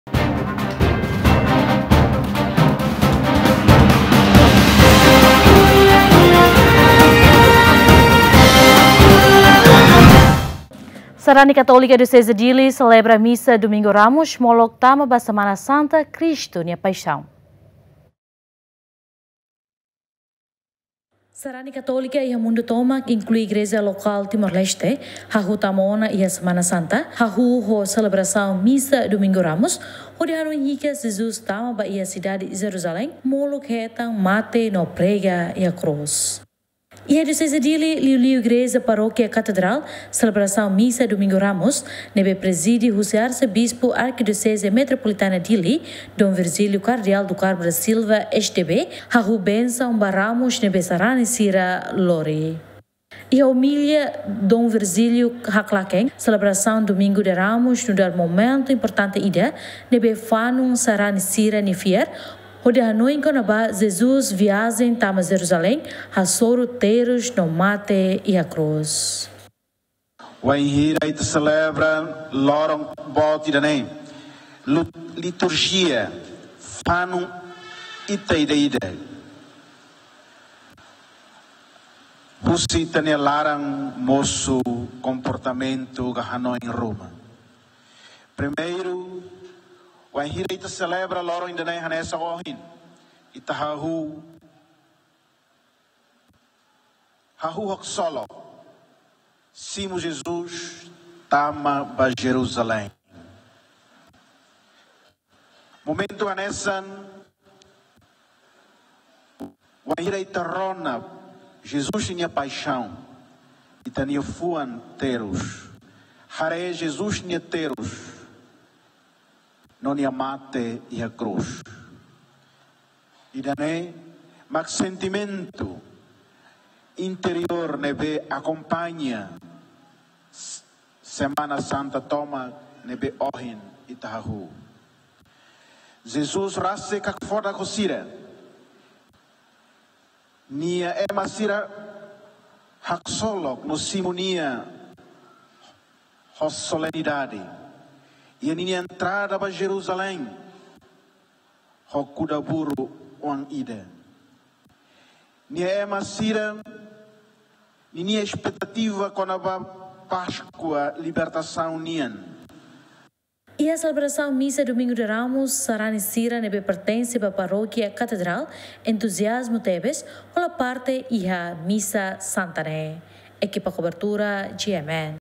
Sanari katólika Dioseze Díli selebra Misa Domingu Ramos molok tama semana santa Kristu nia paisaun
Sanari katólika Dioseze Díli selebra Misa Domingu Ramos molok tama ba semana santa Kristu nia paisaun